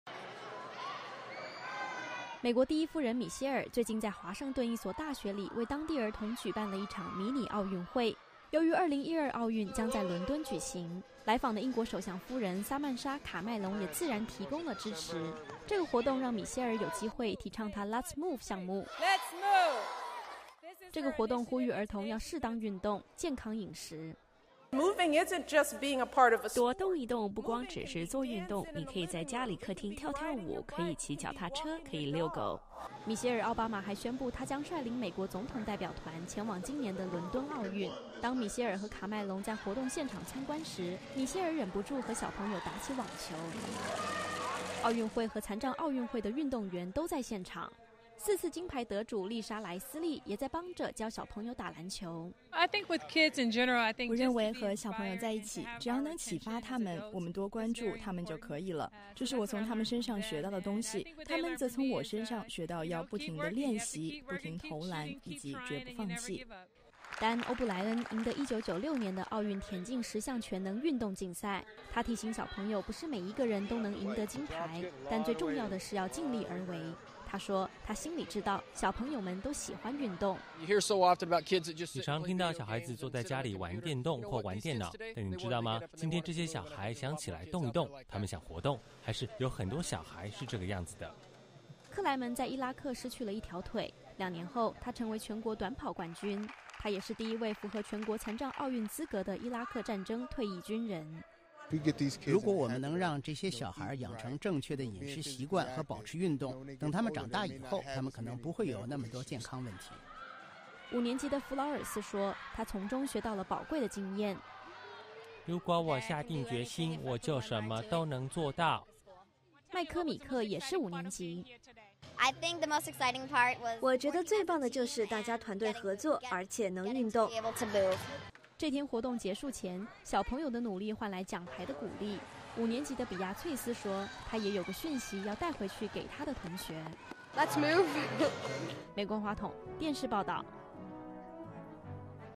美国第一夫人持续推展孩童运动
美国第一夫人米歇尔•奥巴马十分关注儿童健康，她花过去三年的时间宣传运动和健康的重要性。

最近，来访的英国首相夫人也加入了米歇尔的行列，他们两位第一夫人共同出席了一场孩童的运动竞赛。脸书: